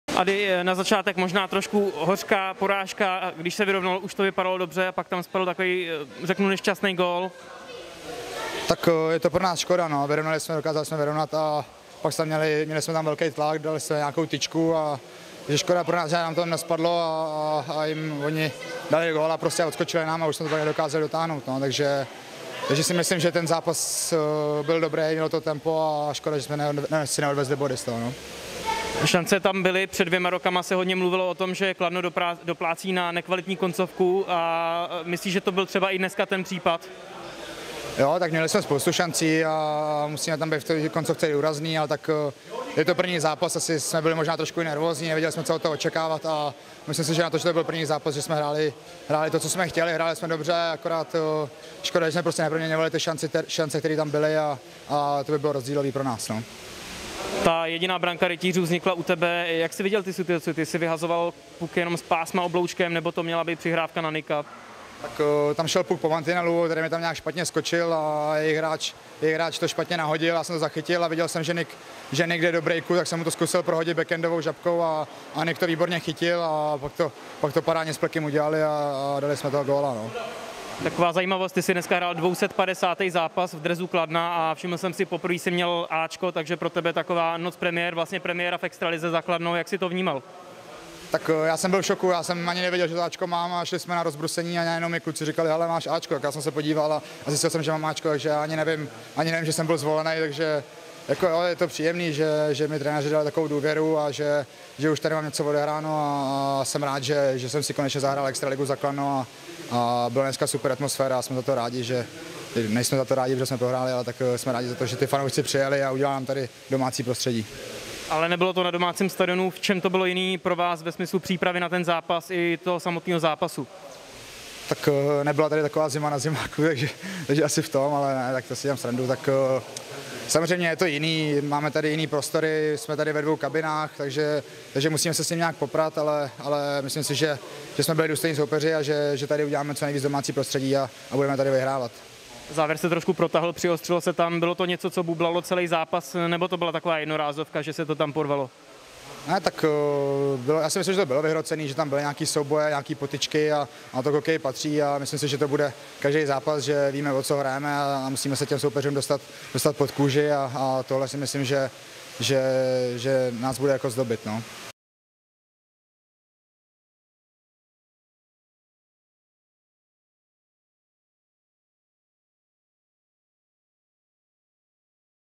Hráli jsme dobře, škoda neproměněných šancí, hodnotí Kubík extraligovou premiéru
V necelých 23 letech za Kladno naskočil už k 250. utkání, poprvé ale v nejvyšší soutěži. Extraligovou premiéru v modrobílém ozdobil Adam Kubík založením akce, na jejímž konci Tomáš Plekanec vyrovnával na 1:1. Na body to ale tentokrát nestačilo.